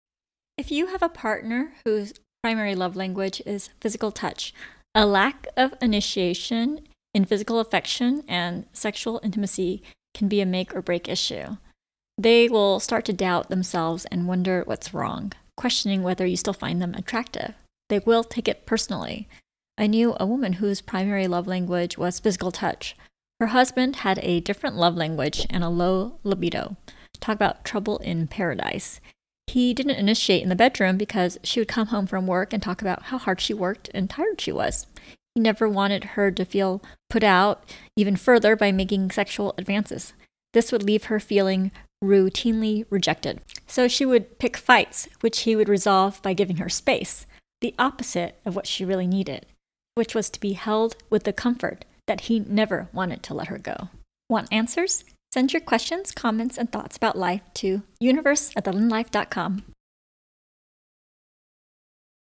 Episode 105 - Rejected Touch
If you have a partner whose primary love language is physical touch, a lack of initiation in physical affection and sexual intimacy can be a make or break issue. They will start to doubt themselves and wonder what’s wrong, questioning whether you still find them attractive. They will take it personally. I knew a women whose primary love language was physical touch. Her husband had a different love language and a low libido. Talk about trouble in paradise. He didn’t initiate in the bedroom because she would come home from work and talk about how hard she worked and tired she was. He never wanted her to feel put out even further by making sexual advances. This would leave her feeling routinely rejected. So she would pick fights which he would resolve by giving her space. The opposite of what she really needed. Which was to be held with the comfort that he never wanted to let her go.